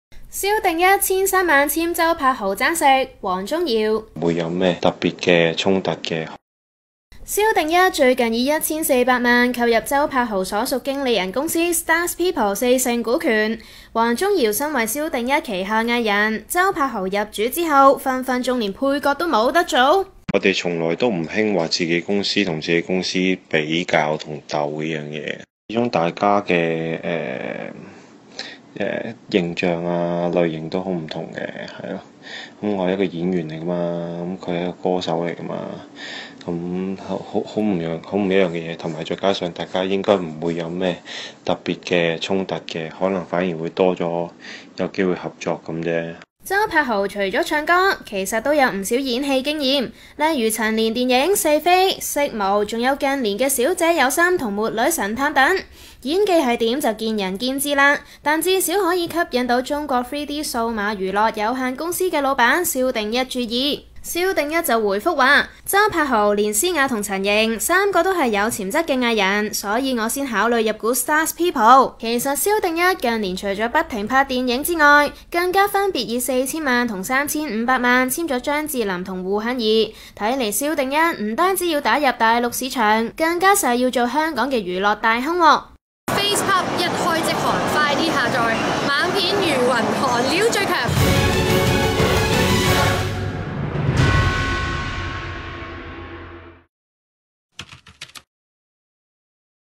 蕭定一1300萬簽周柏豪爭食 王宗堯：唔會有特別衝突
蕭定一自從01年踩隻腳入娛樂圈後，業務真係蒸蒸日上，除咗不停拍電影之外，佢仲用咗近億簽張智霖和胡杏兒做佢旗下藝人，早前又同靚模公司GME合併，周秀娜、Jeana都收歸旗下，原來蕭定一為咗擁有周柏豪，上月底用咗一千三百萬買咗starz people四成股權，睇嚟「中國3D數碼娛樂有限公司」追擊緊同樣有龐大資金嘅「太陽娛樂」喎！